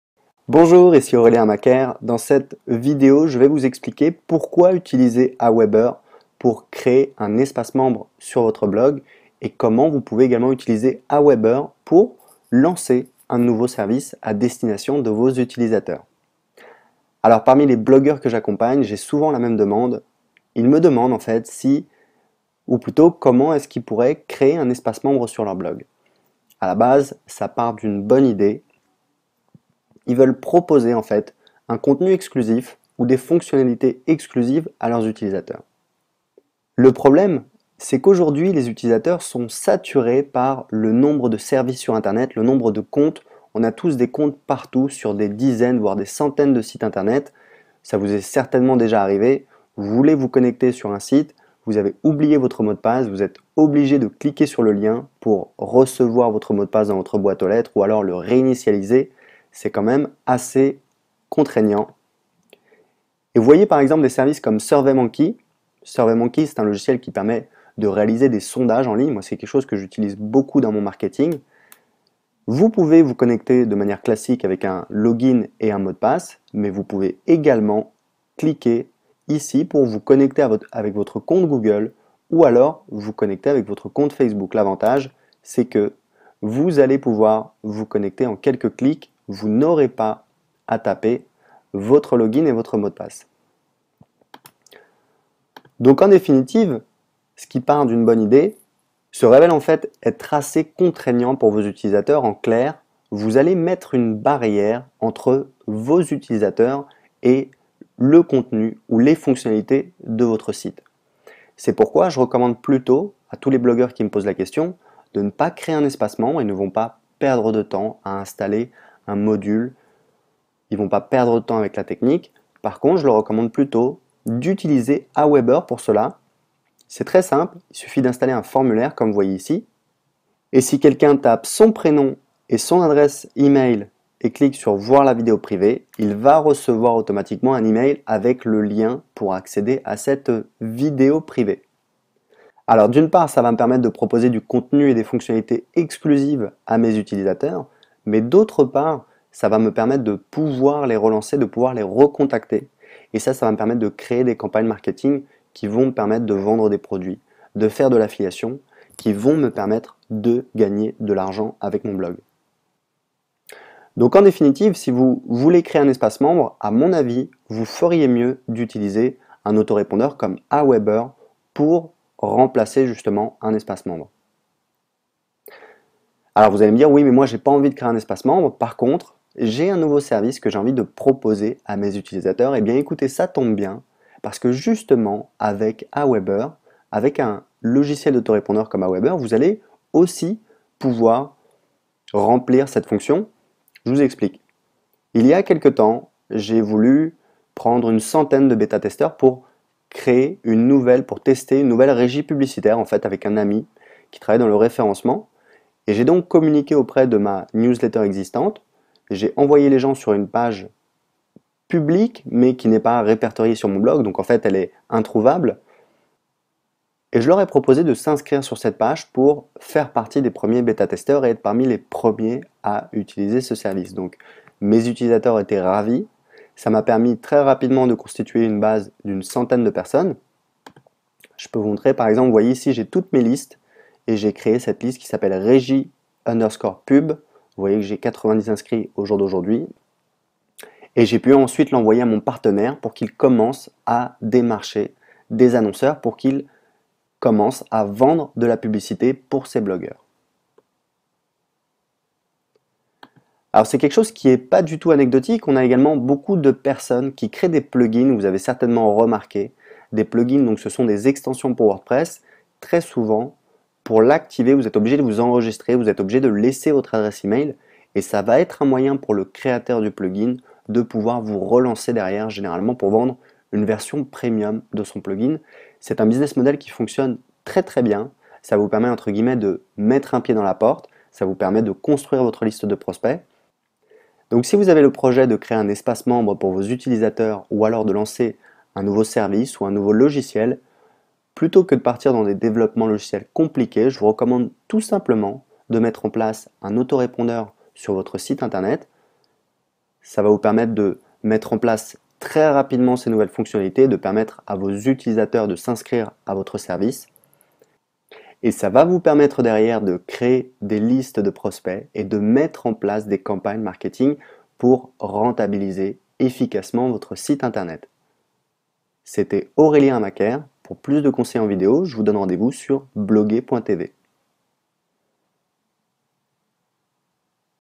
Comment créer un espace membre avec Aweber
Dans cette vidéo d'explique pourquoi vous ne devez pas créer d'espace membre et comment vous pouvez utiliser Aweber à la place.